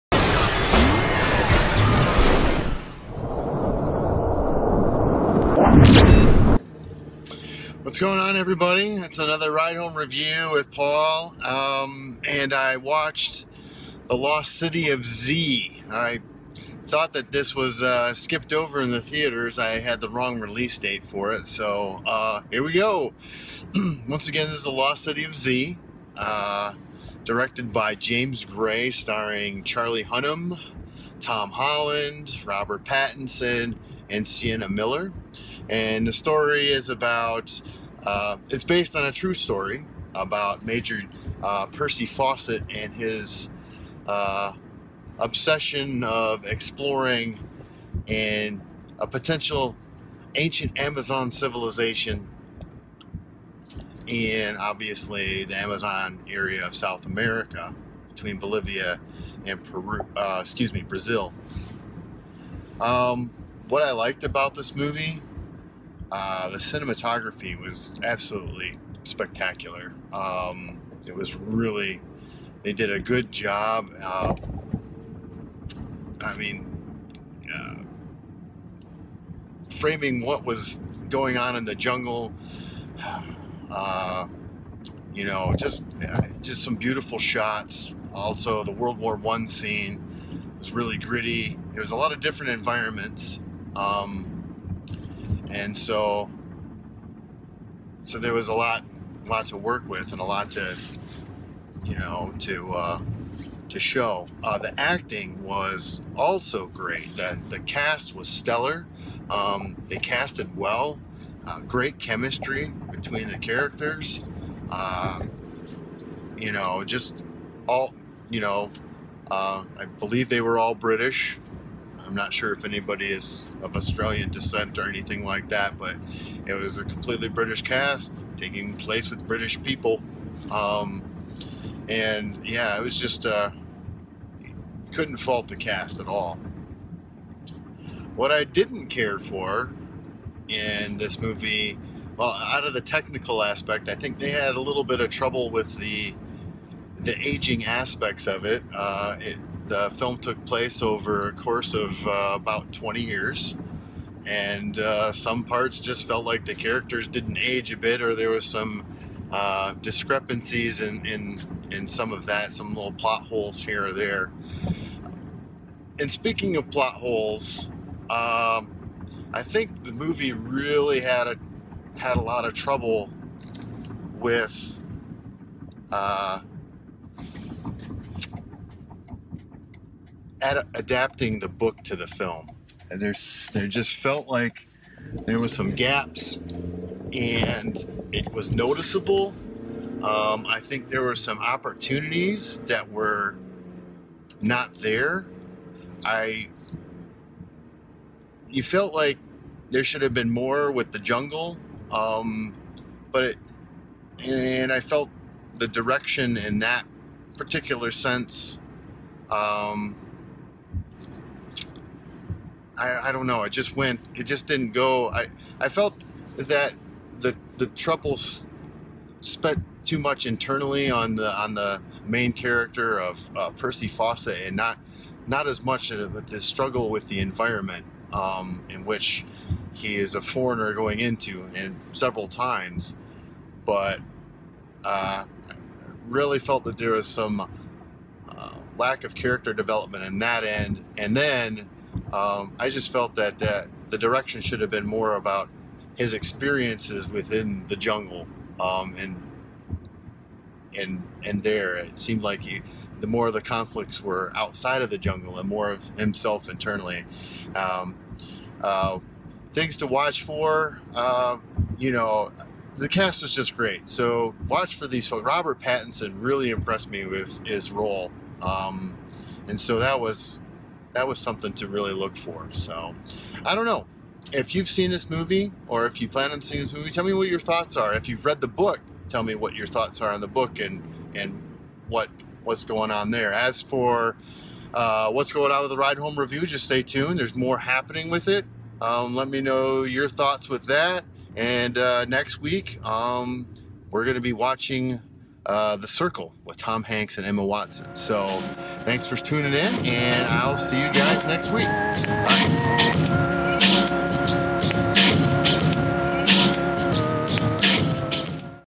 RideHomeReview - The Lost City of Z
A true-life drama, centering on British explorer Col. Percival Fawcett, who disappeared while searching for a mysterious city in the Amazon in the 1920s.

Director: James Gray
Writers: James Gray (written for the screen by), David Grann (based on the book by)
Stars: Charlie Hunnam, Robert Pattinson, Sienna Miller